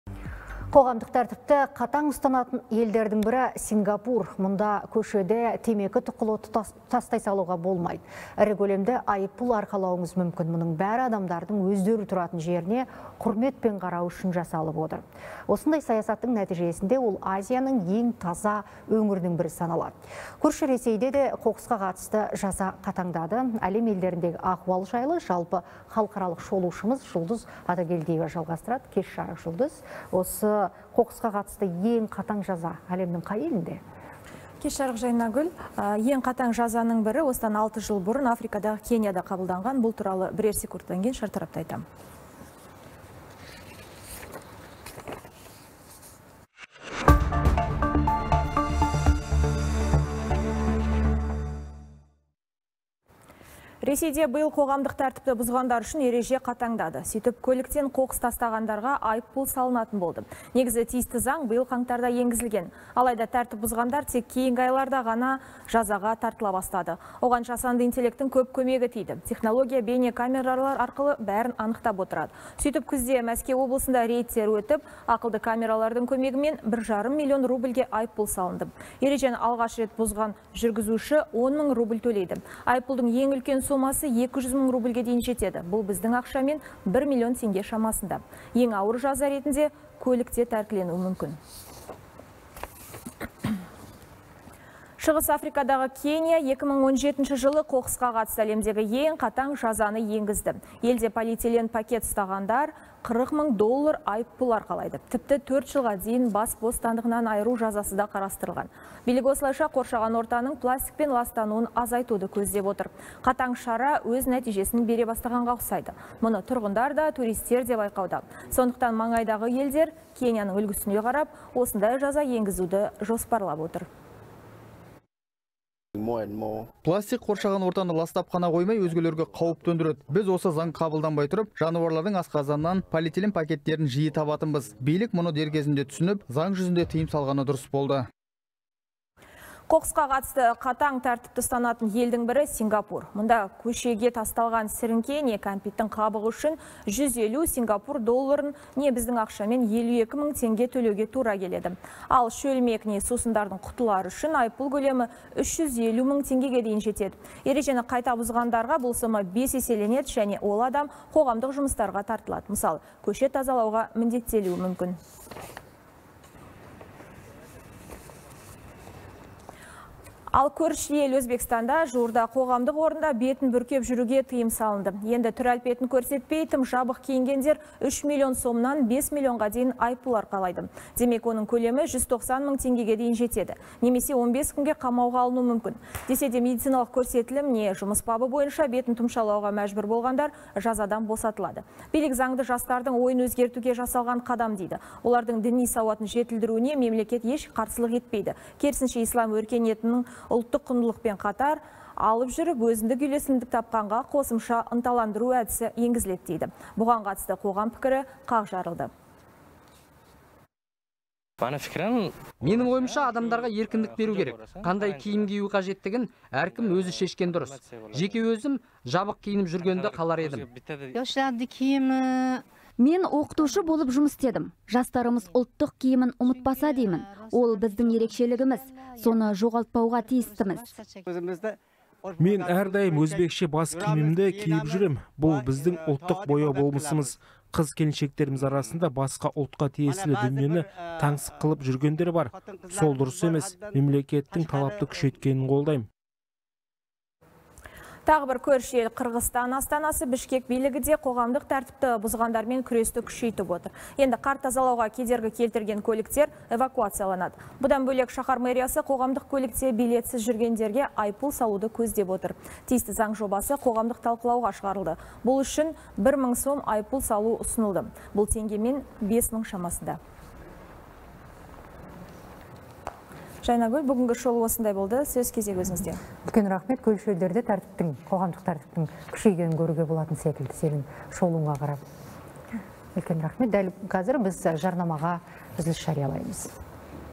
Өзбекстанда қоғамдық орында бетті бүркеп жүруге тыйым салынды
Қоғамдық тәртіпті қатаң ұстанатын елдің бірі – Сингапур.